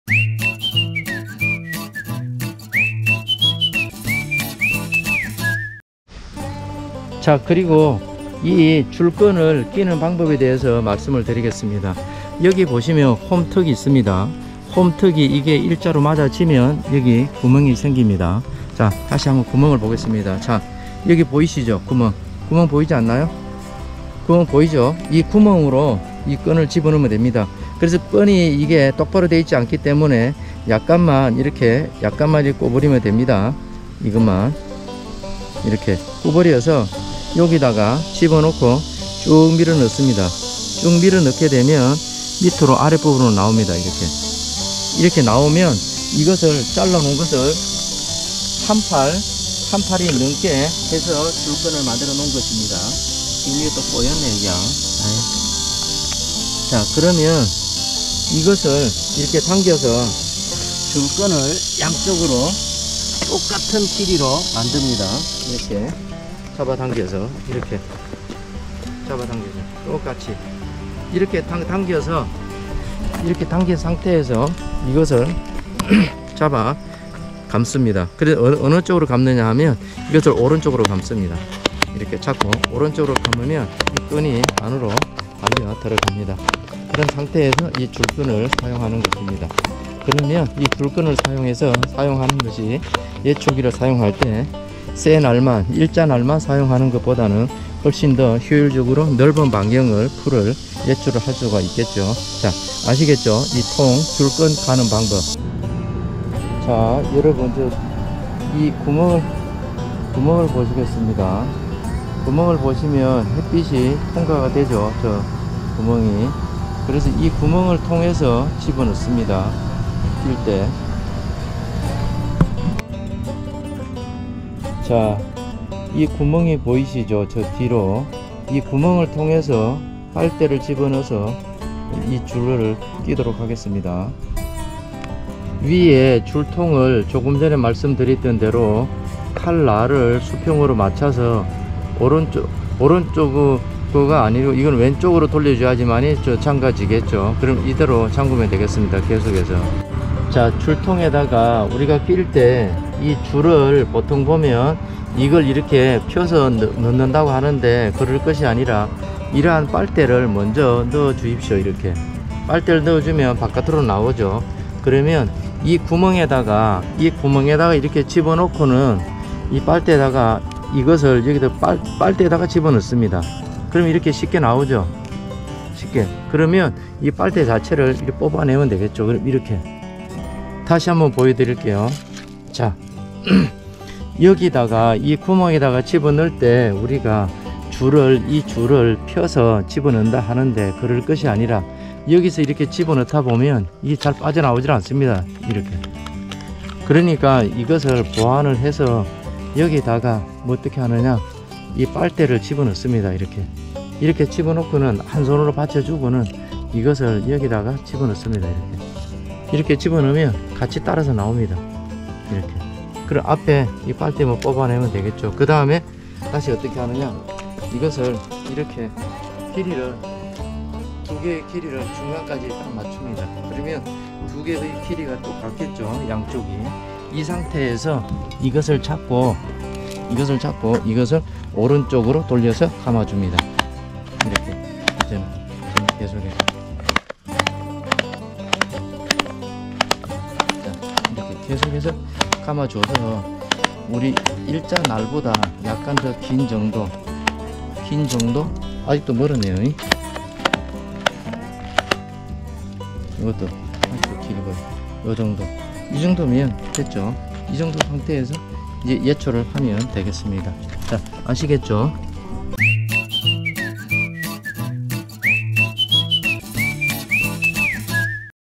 [예초기 활용 시리즈 4/5] 누구나 쉽게 줄끈 끼는 꿀팁! 20230913
풀과의 전쟁에서 풀을 제어하기 위한 예초작업에서 자주 활용되는 것은 예초기입니다.
2도날을 사용하여 주로 예초작업을 하지만 줄끈을 활용하면 넓은 반경의 작업 뿐만 아니라 돌이나 쇠붙이 등이 있는 주변의 예초작업을 할 때 매우 유용한 방업입니다.

그러나 줄통 이용하려면 줄끈 쉽게 끼어야 하는데 여간 어려운 일이 아닙니다.
따라서 예초기의 줄통에 누구나쉽게줄끈끼는꿀팁! 알려드리겠습니다.

이러한 안내가 "줄끈 끼는 꿀팁! "에 대한 정보공유 차원에서 조금이나마 도움이 되었으면 합니다.

[Tag|태그]

2021년 9월부터 유튜브를 제대로 하자고 마음먹은 초짜유튜버 입니다.
감사합니다.